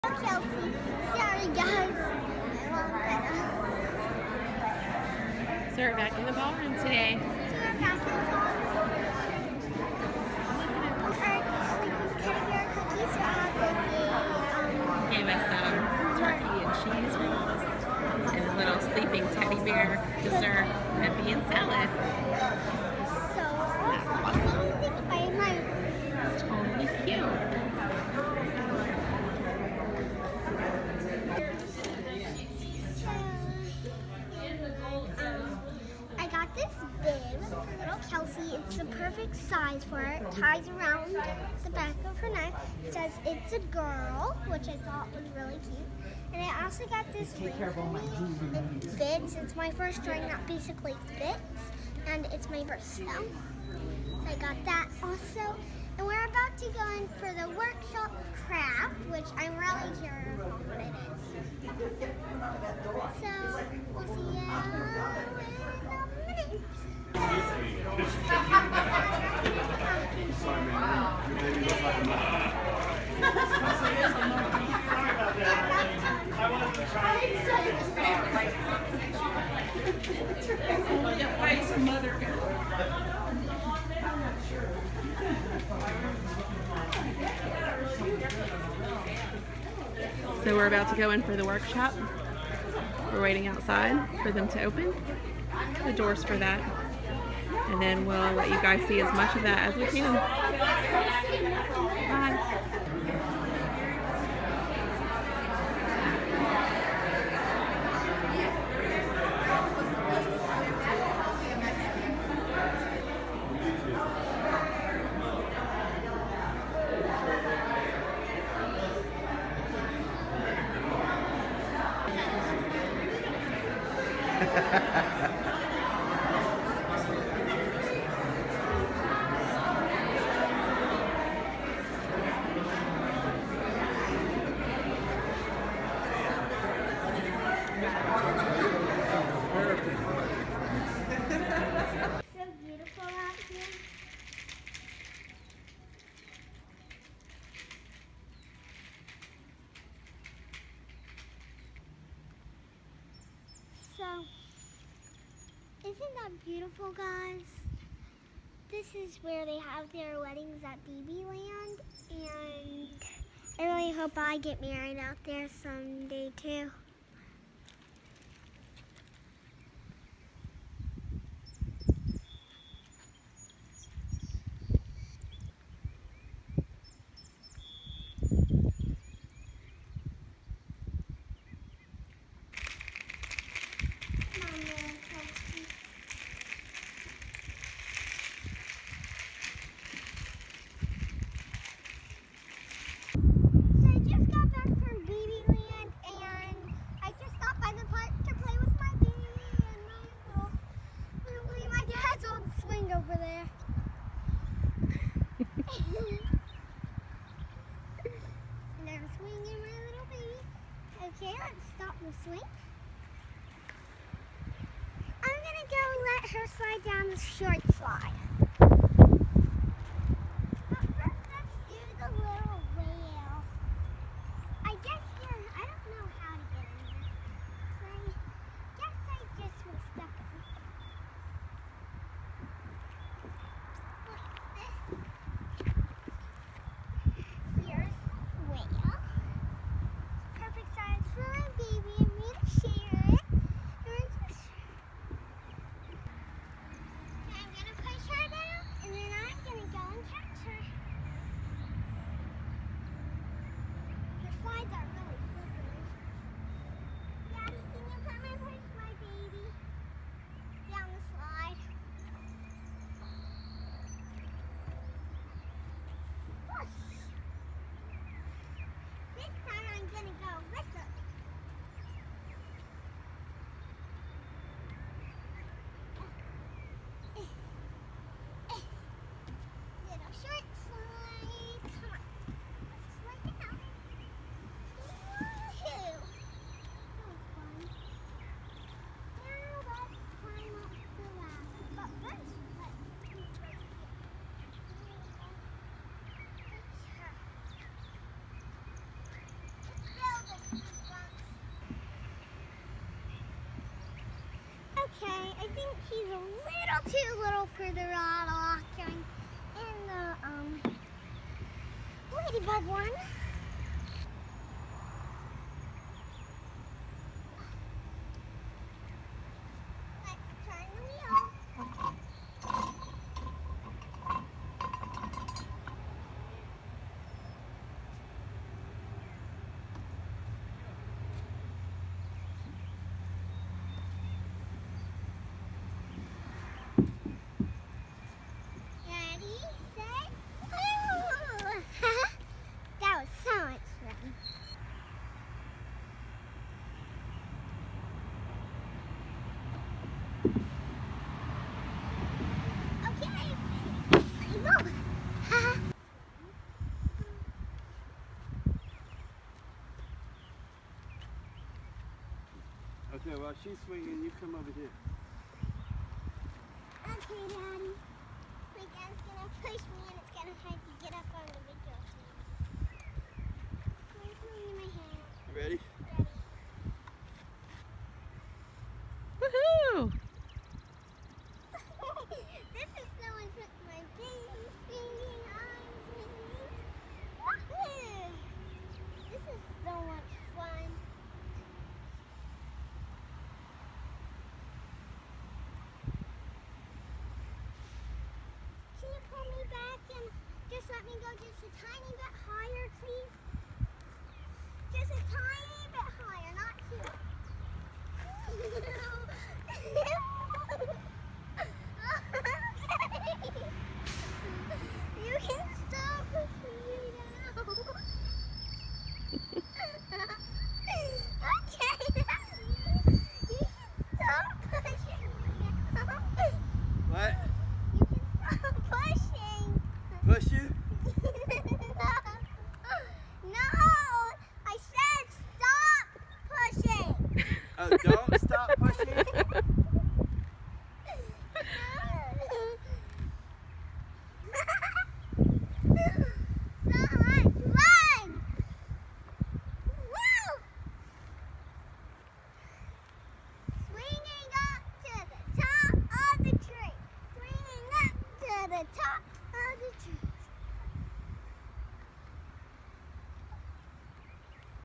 Day 2 At Babyland CPK SE And Fun At The Park! by New Toy Collector Family
Collector's Club Spring Event at Babyland General Hospital
Fun at the park with Cabbage Patch Kids Preemie.